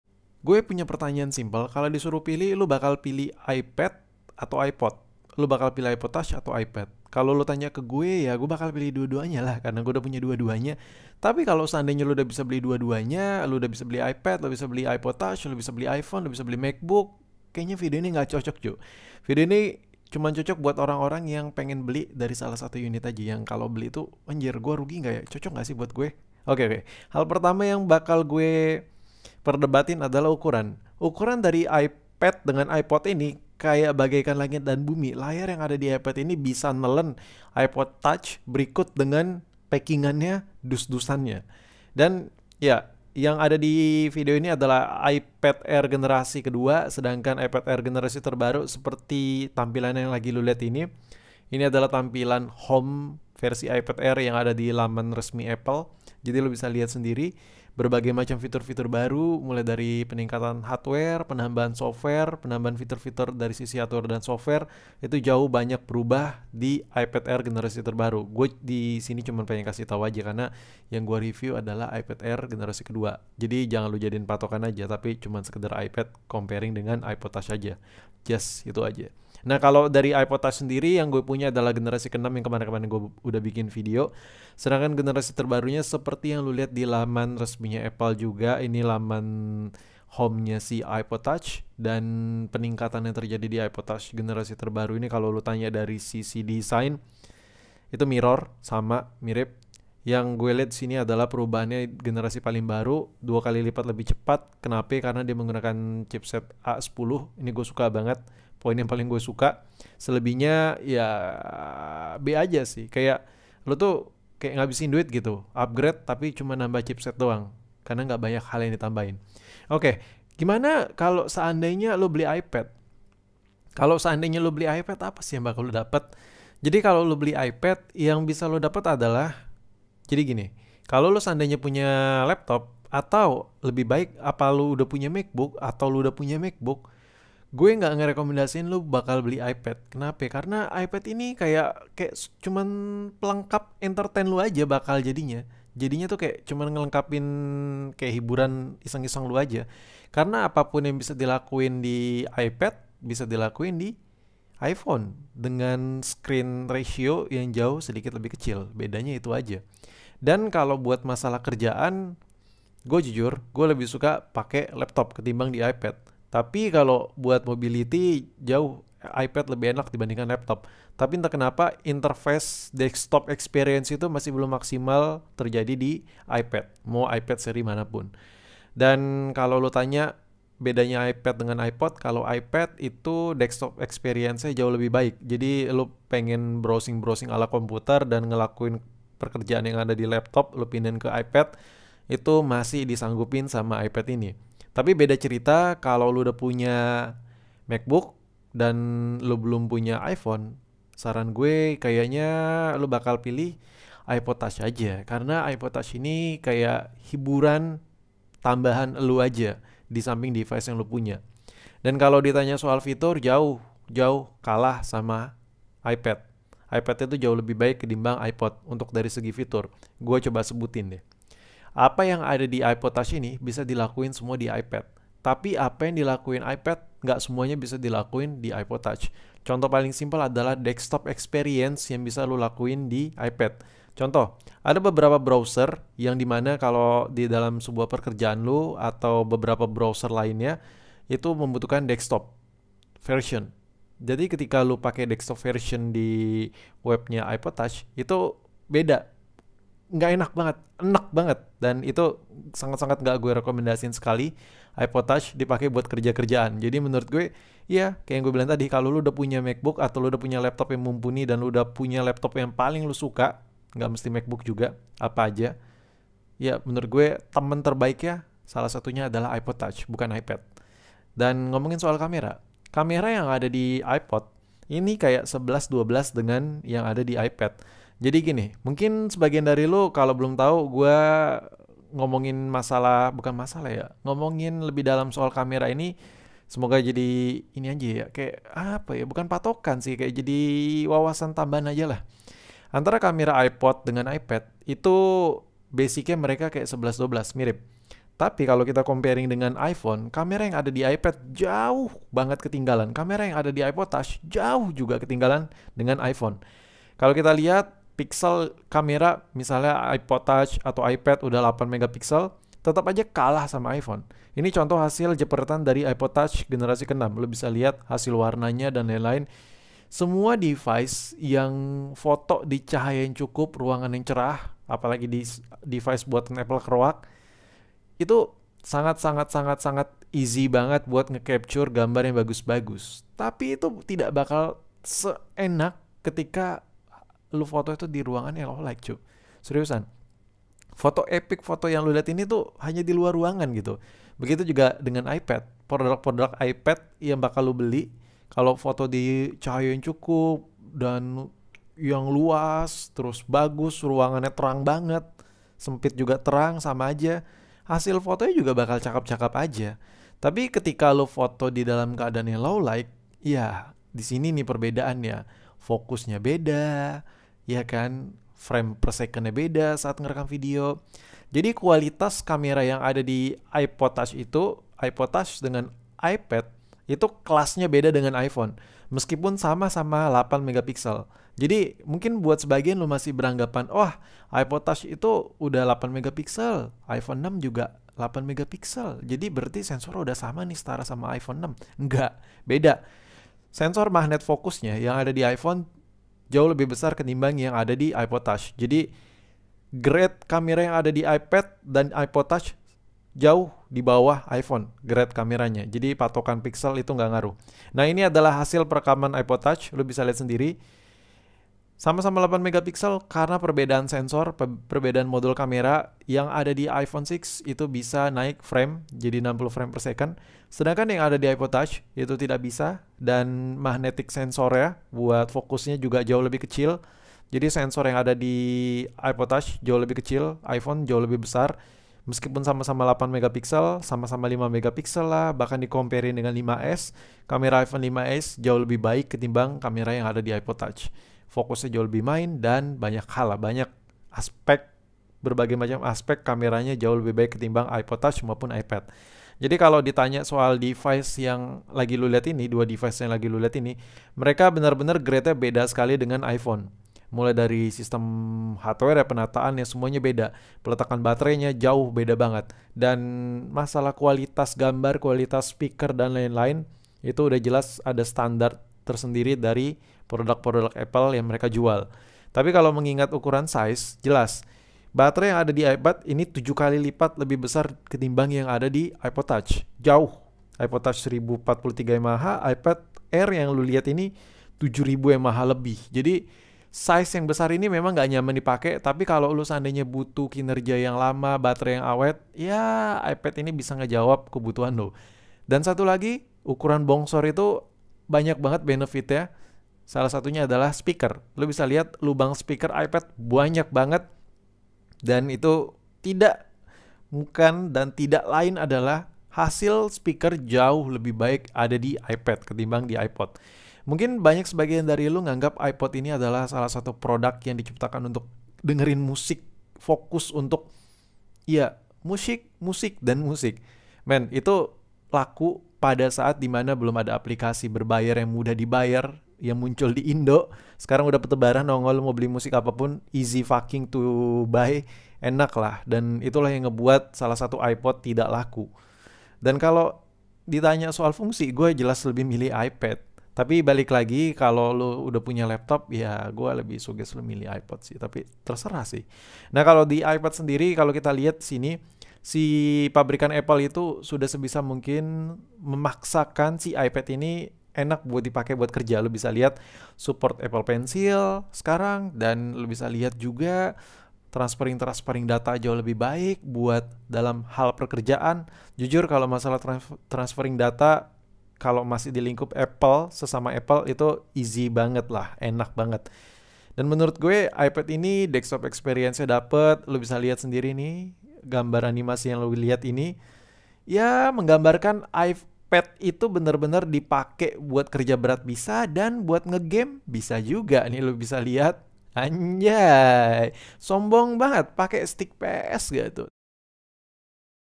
Kalau dikasih Gratis, mending Pilih iPad atau Pilih iPod Touch?
Keunggulan iPad;
-bisa ganti in kerja laptop dikit dikit
-di pake buat nonton enak, layar besar
-face cam buat meeting kerjaan nyaman
-baterai besar
-suara speaker besar
-buat nge game nyaman
-buat ngedit,video,foto cocok

Keunggulan iPod touch;
-ukuran nya kecil (kek punya lu)